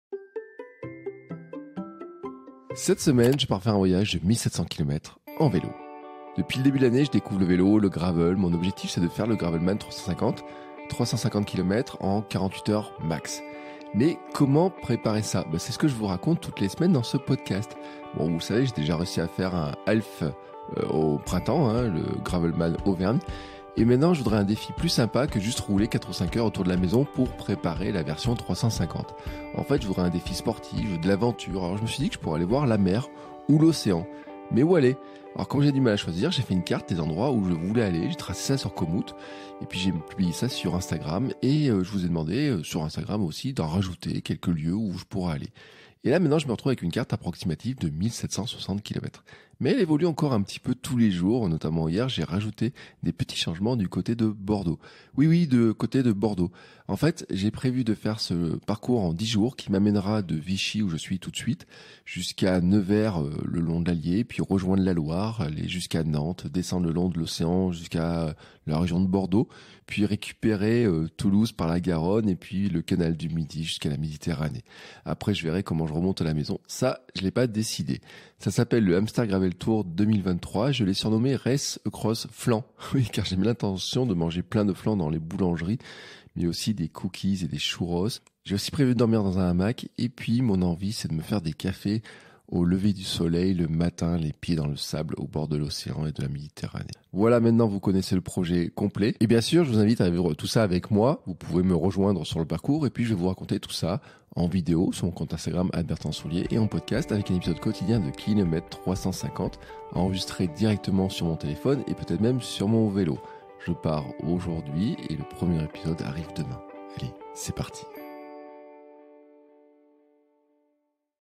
Hamster Gravel Tour 2023
Cette semaine je pars faire un voyage de 1700 km... à vélo.

Depuis le début de l'année je découvre le vélo et le gravel et je vous raconte tout ça dans le podcast.

Pour préparer mon Gravelman je n’étais pas très motivé par des sorties de 4 ou 5 heures autour de la maison. J’ai envie d’aventure et d’un défi plus sportif. Alors j’ai chargé mes sacoches sur mon gravel et je pars. D’abord en direction de Nevers pour rejoindre la Loire jusqu’à Nantes.

Puis longer l’océan Atlantique jusqu’à Bordeaux ou un peu plus loin. Et ensuite bifurquer vers Toulouse et aller récupérer le canal du Midi jusqu’à la Méditerranée.Je vais vous raconter tout ça en vidéo sur mon compte Instagram @BertrandSoulier ainsi que sur ce podcast KM350 avec un épisode quotidien.